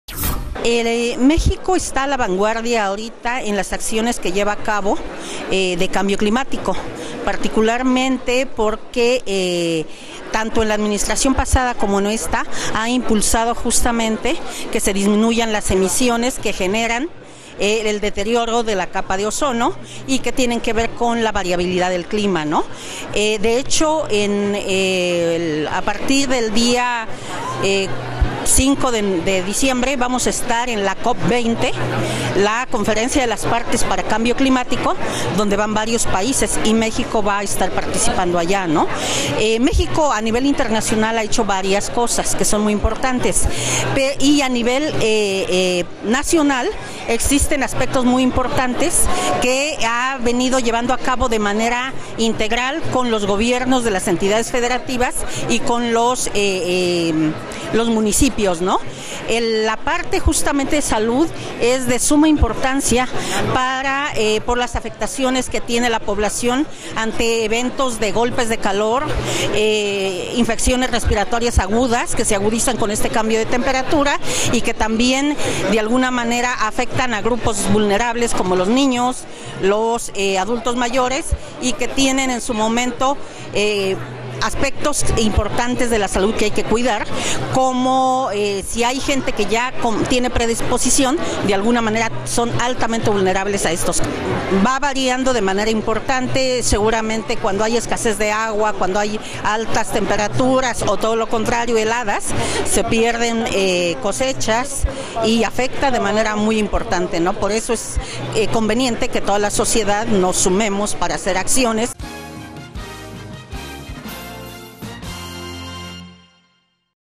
Cambio Climático tiene alta incidencia en enfermedades respiratorias
México se encuentra a la vanguardia al realizar acciones contra el cambio climático, particularmente porque se ha impulsado que disminuyan las emisiones que se generan en deterioro de la capa de ozono y que tienen que ver con la variabilidad del clima, señalo Guadalupe de la Luz González, subdirectora ejecutiva de Valuación de Impacto Ambiental de la COFEPRIS. Abundó que el cambio climático tiene una importante incidencia en infecciones respiratorias agudas, dengue, paludismo y Chagas.